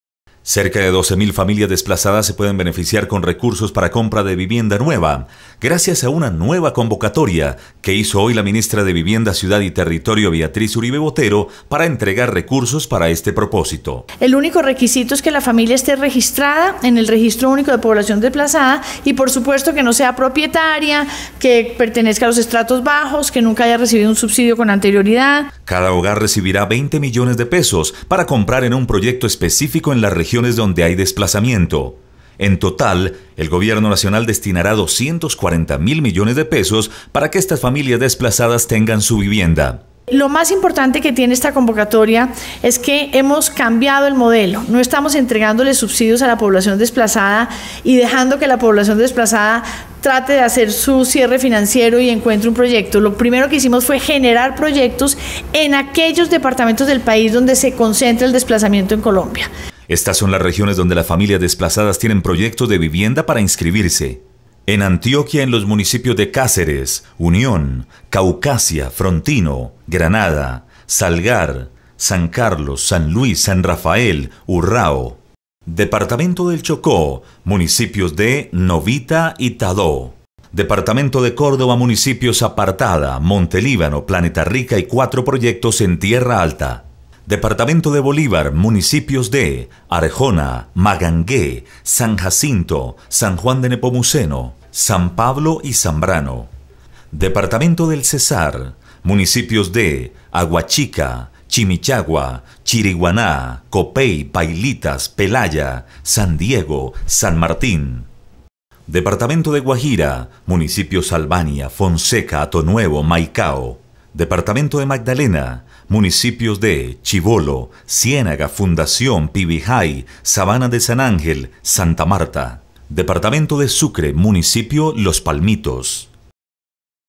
Convocatoria para entrega de recursos a población desplazada.
Ministerio de  Vivienda,  Ciudad y Territorio,  abrió una nueva convocatoria para entregar recursos para compra de vivienda nueva a población desplazada.